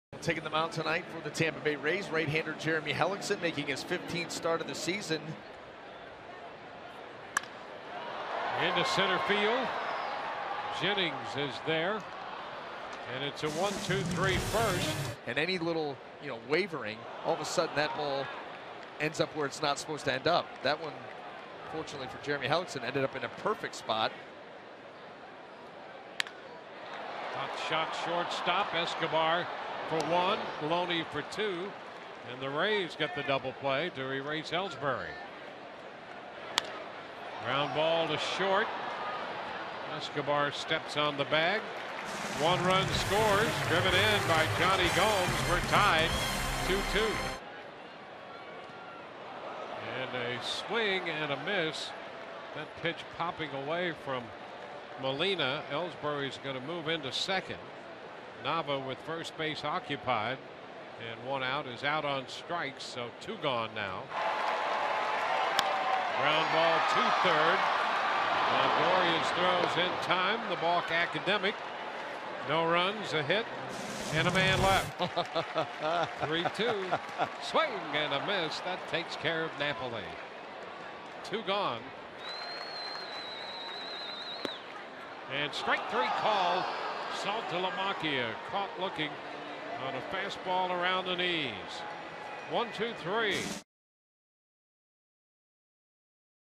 2013/06/19 Hellickson's solid start
TB@BOS: Hellickson allows two runs, fans four in win